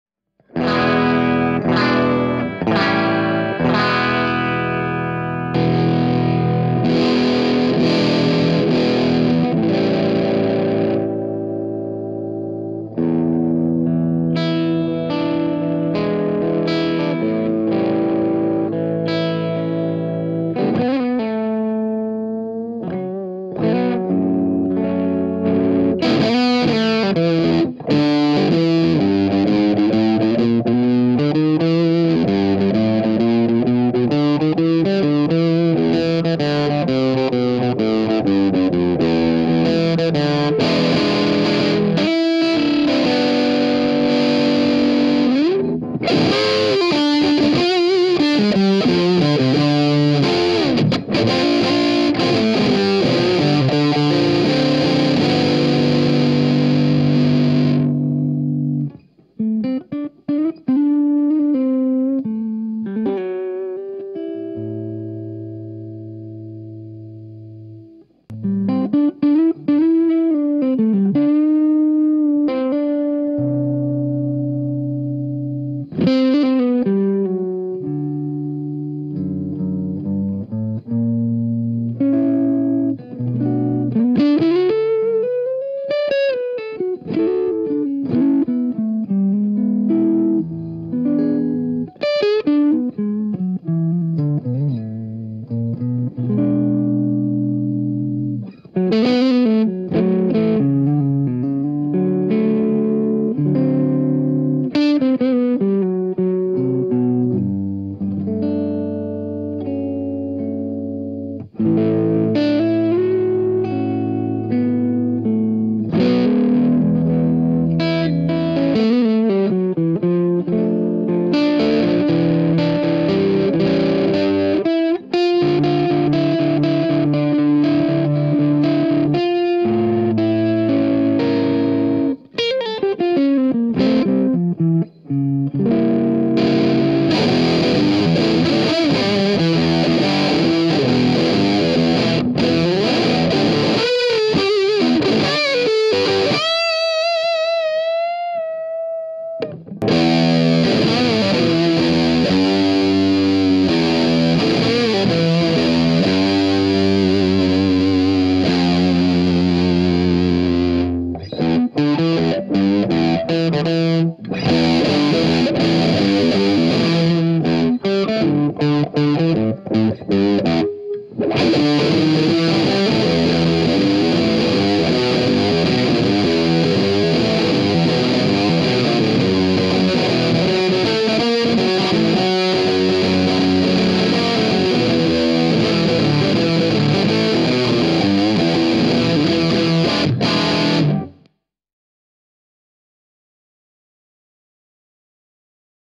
Mad Professor Fire Red Fuzz
Abbiamo provato con Michele Quaini e la sua Marshall Plexi un fuzz sfizioso, facile da usare e con un sacco di sustain, il Fire Red Fuzz, della Mad Professor. Tre controlli e tanta cattiveria, facile da calmare con la manopola del volume. _______________